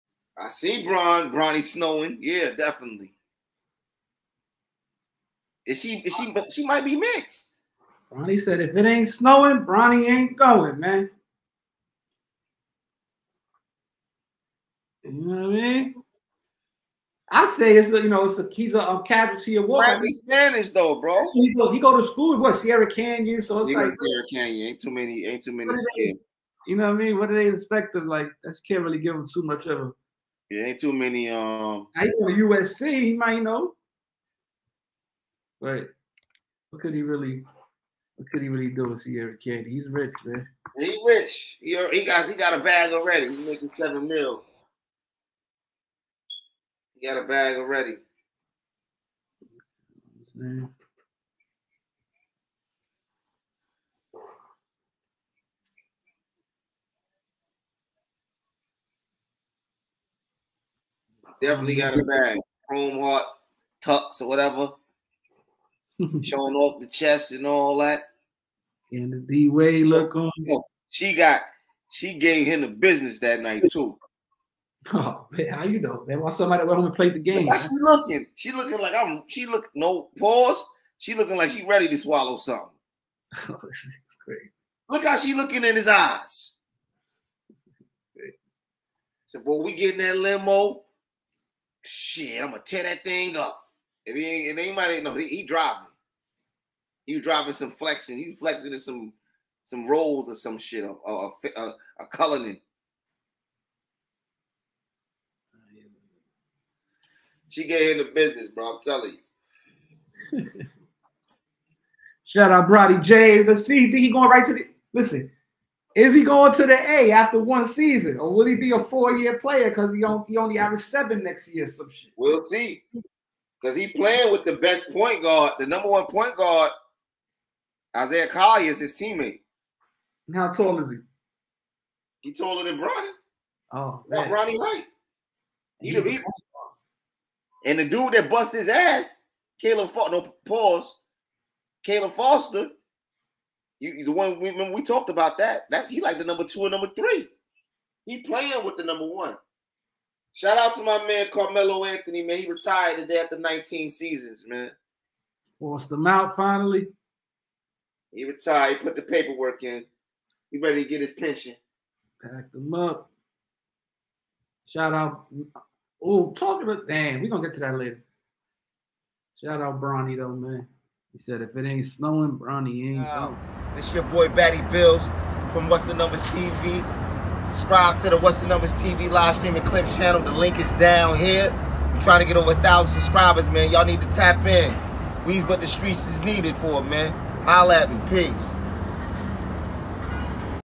Bronny James Gets More Nasty Comments For Bringing White Girl Back To Prom..
Poro & Batty Billz Talk About Bronny James Recent Prom Pic..